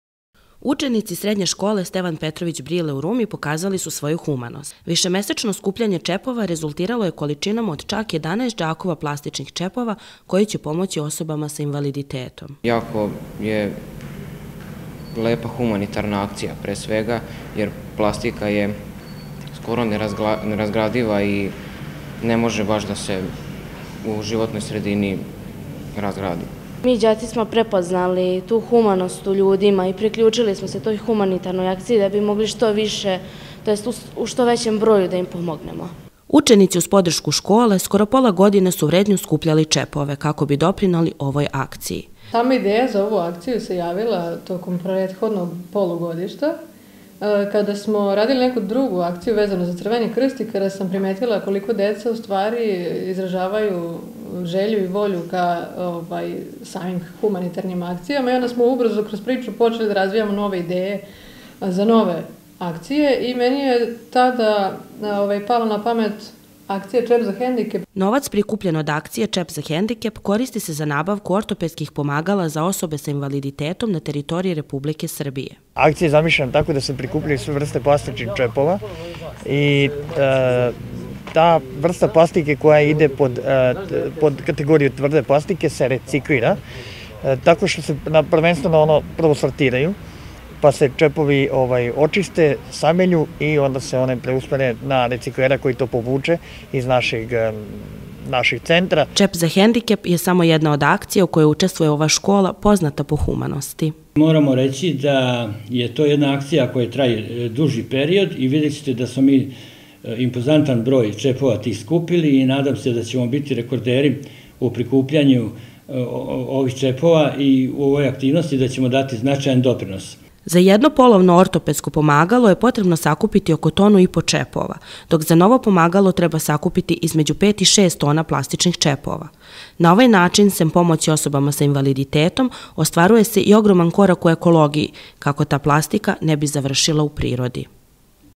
Zvanično preuzeti sakupljenih čepovi za humanitarnu akciju "Čep za hendikep" u Rumi
Srednja poljoprivredno - prehrambena škola ,,Stevan Petrović Brile" iz Rume danas je organizovala preuzimanje čepova koji su sakupljeni u sklopu humanitarne akcije ,,Čep za hendikep". Ova humanitarna akcija organizuje se širom Srbije već nekoliko godina u nazad sa ciljem da pomogne osobama sa invaliditetom, kupovinom ortopedskih pomagala.